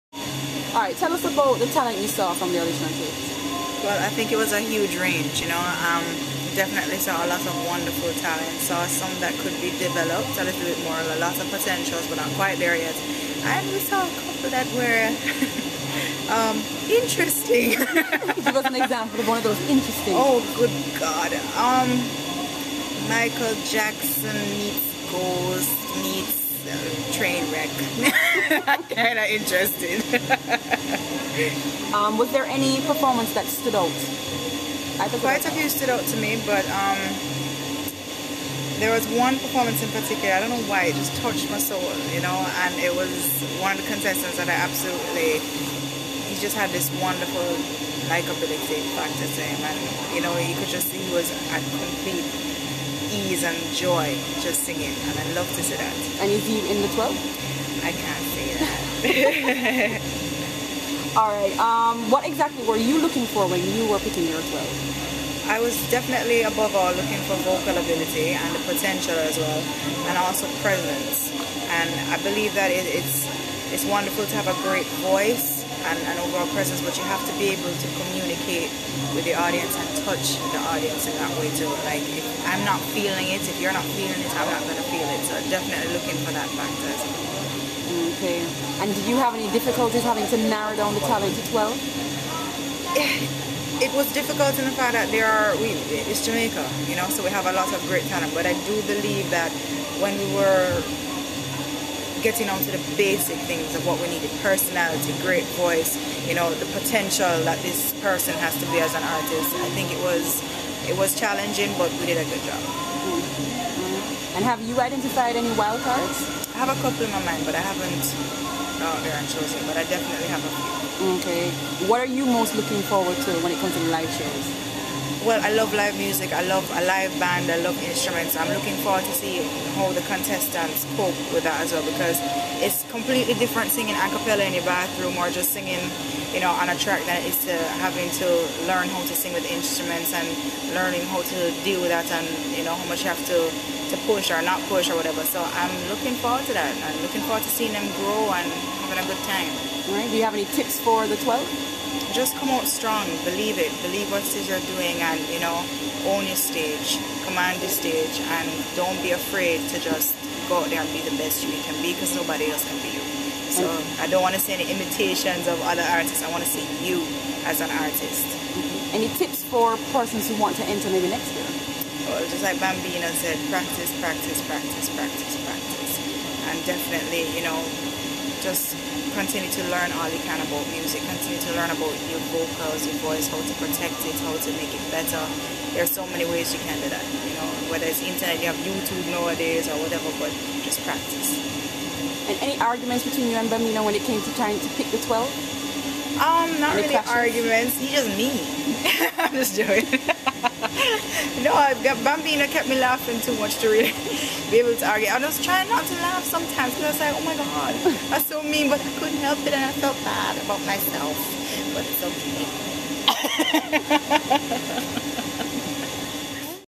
Tessanne talks about Digicel Rising Stars
We caught up with one of the ALL NEW DIGICEL RISING STARS JUDGES for 2011, Tessanne Chin to see what it was like selecting the TOP 12.